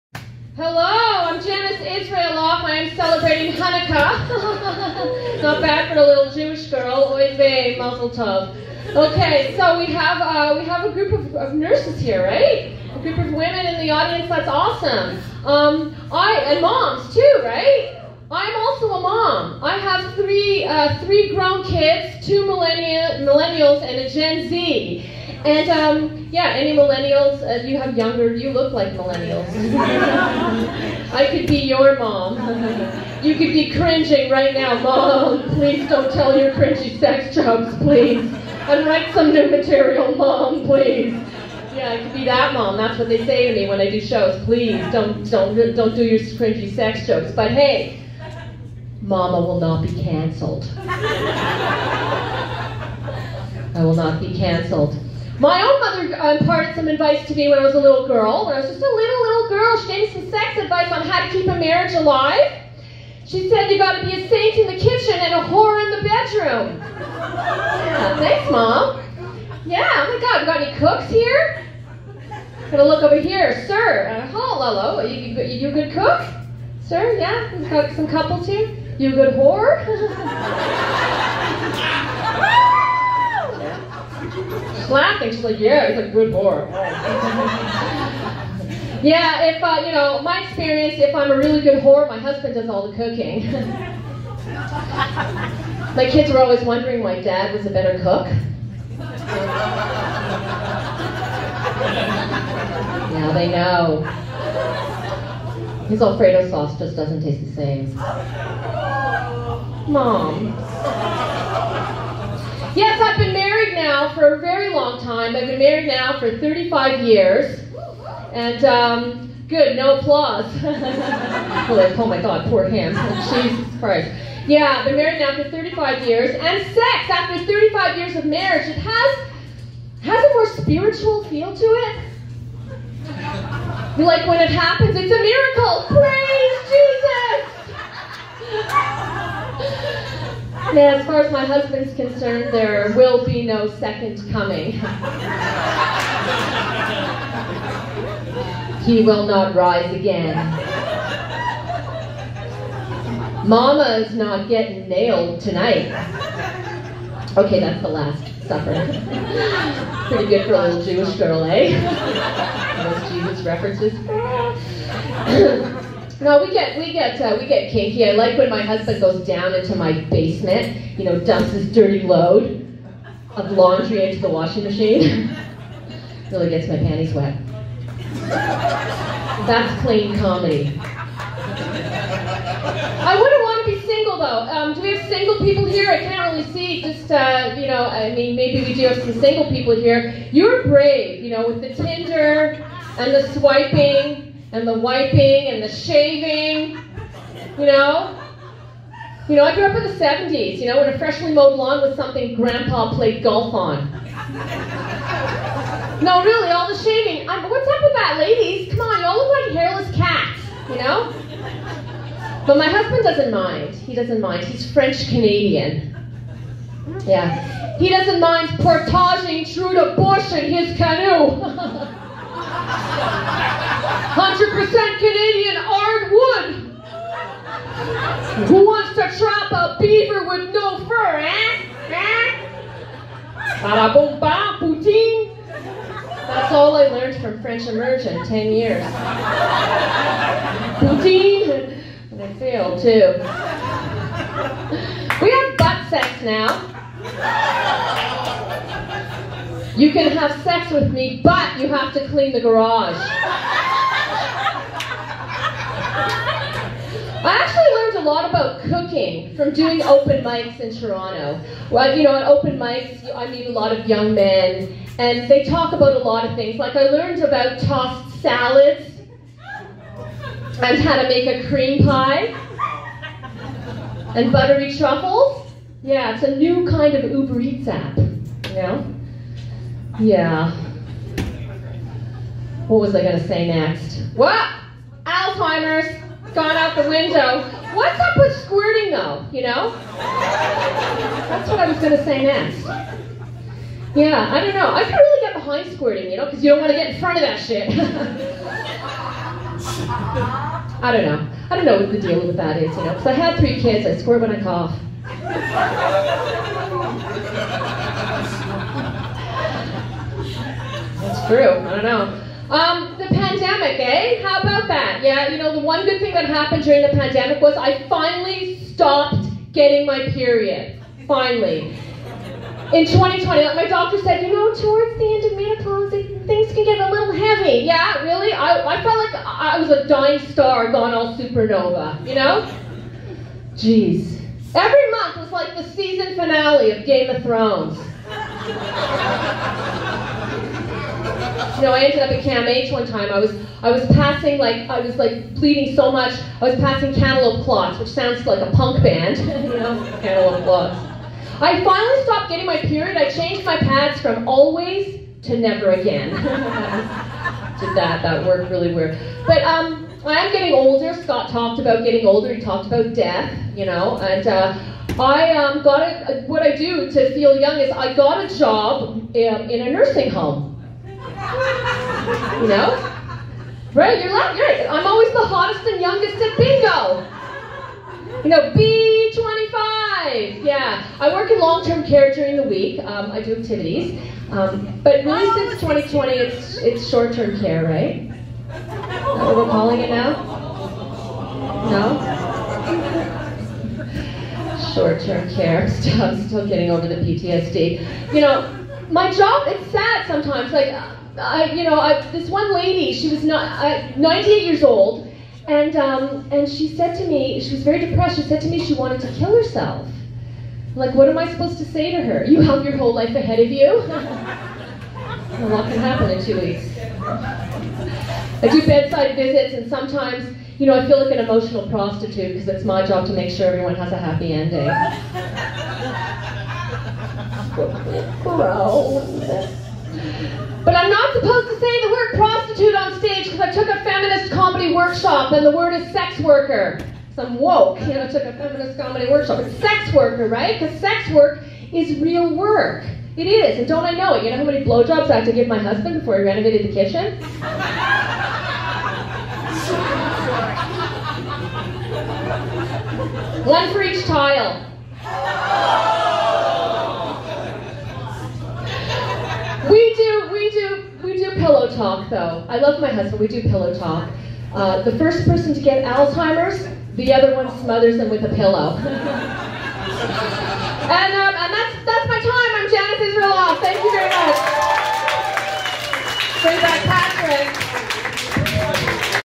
Levity Dec 2021
December 1 2021 performance at Levity Comedy Club in Hamilton, Ontario.